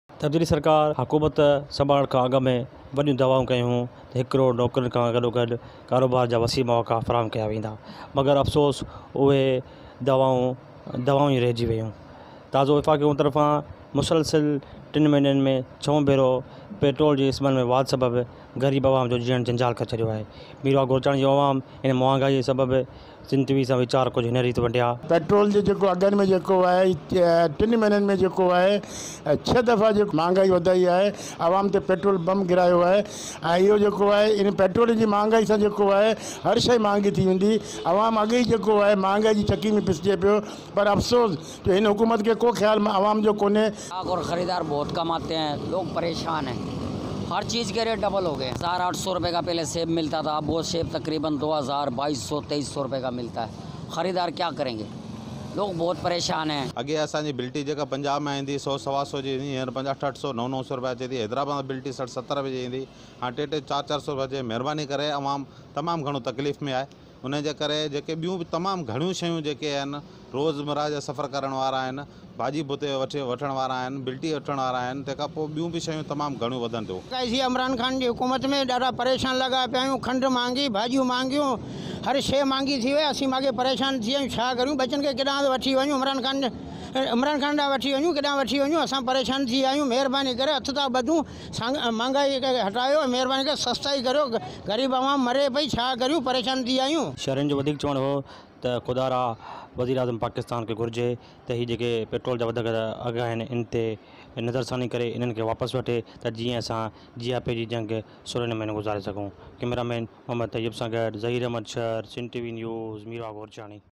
Petrol Mahango: Mirwah Gorchani mein Aam Wahipay wariyun shayun jay aghan mein Wadh
Sindh TV News is the "First Ever Sindhi News Channel" providing its viewers latest International,National,Regional, Sports and Showbiz News.